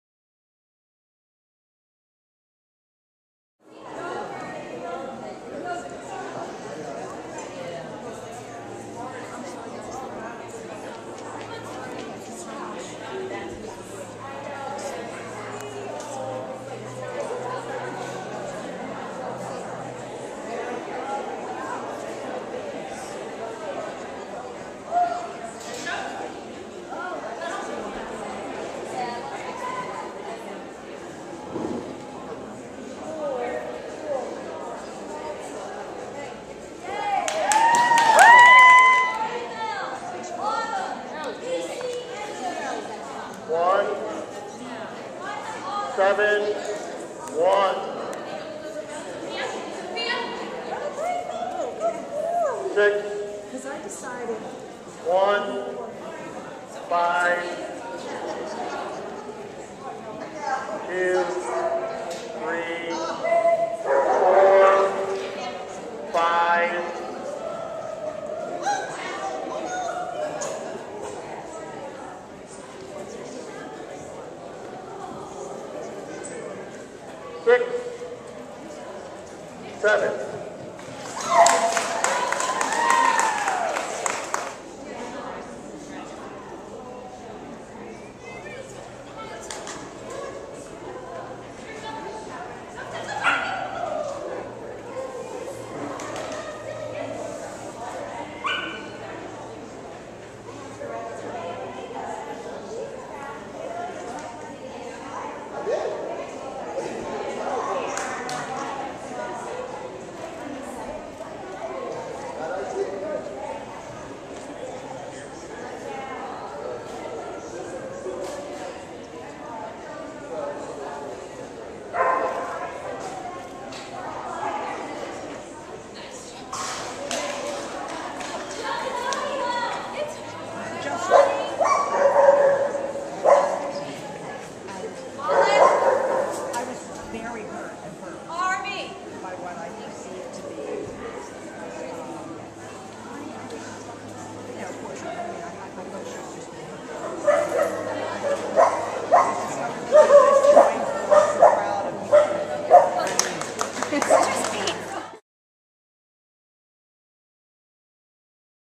Y2K9's CPE Trial - September 18-19, 2010
This past weekend, y2k9's hosted their annual CPE trial! It was a great opportunity to try some new handling moves (practiced lots of rear crosses) and we had a great time.